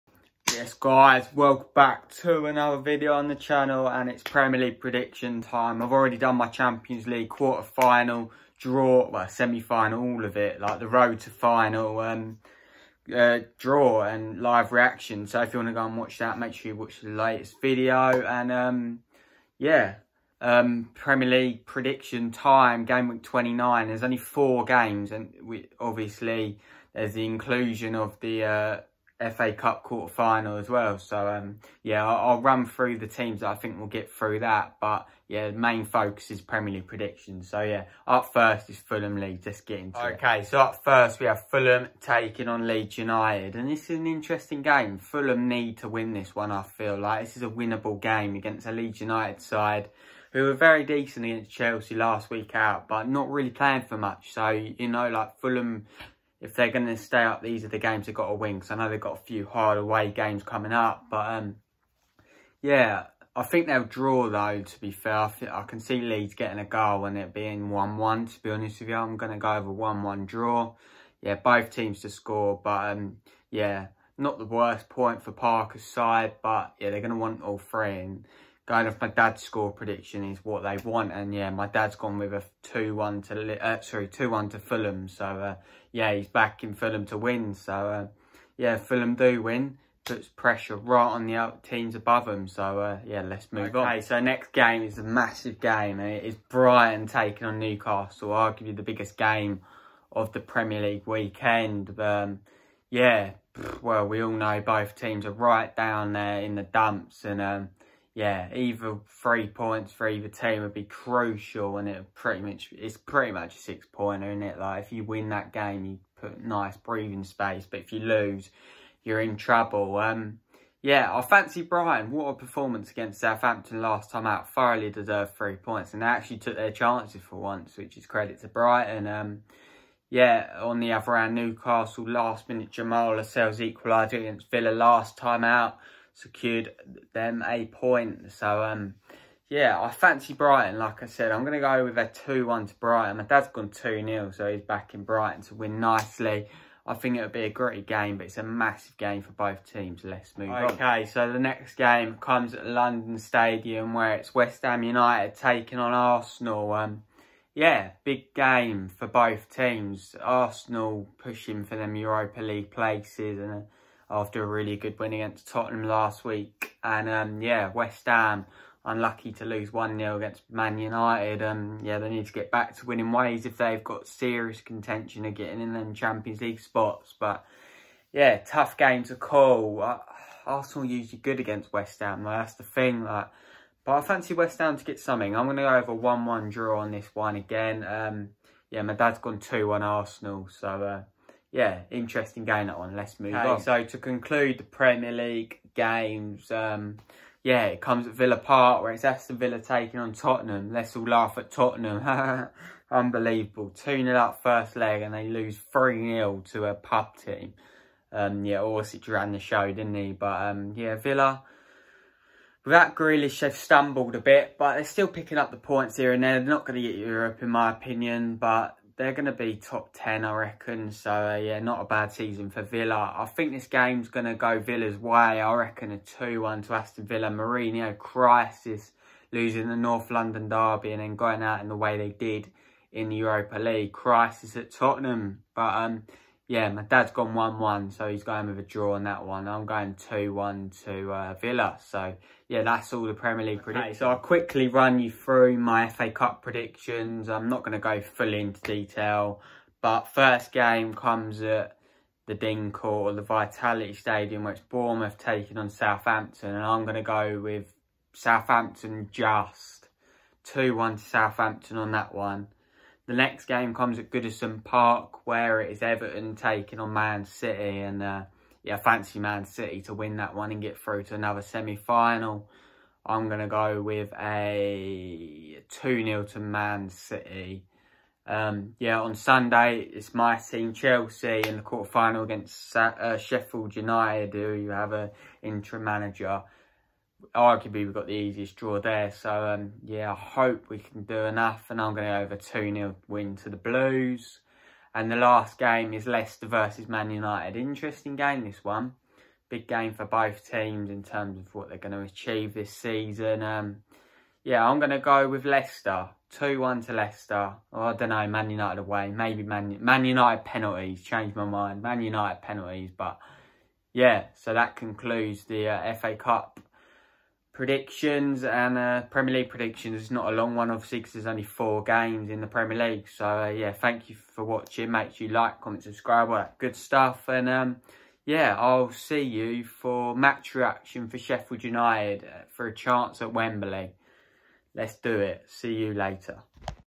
PREMIER LEAGUE PREDICTIONS GAMEWEEK 29 /FA CUP PREDICTIONS- BIG GAMES ALL ROUND!!!
Our FA Cup & Gameweek 29 Predictions: Leicester vs Man United, Everton vs Man City, Aston Villa vs Tottenham, West Ham vs Arsenal, Bournemouth vs Southampton, Fulham vs Leeds, Chelsea vs Sheffield Utd and Brighton vs Newcastle...